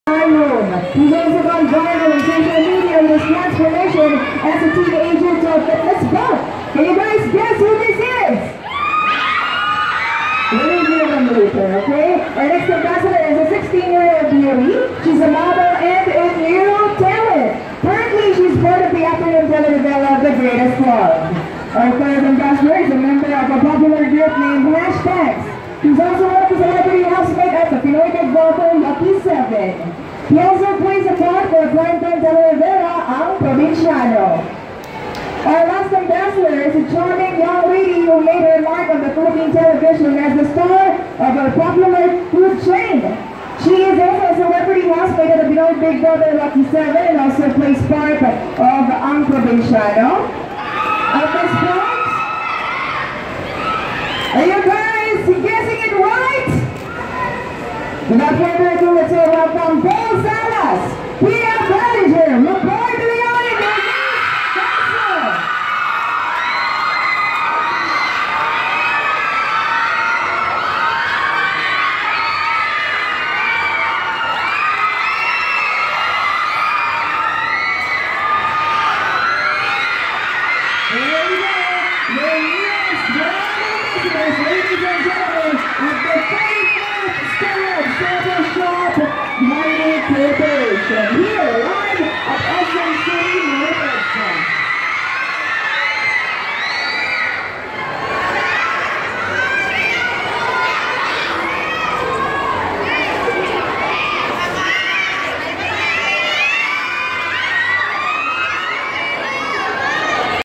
Welcome Surplus brand ambassadors november 2016
Welcome new Surplus brand ambassadors during SM Annex Surplus opening.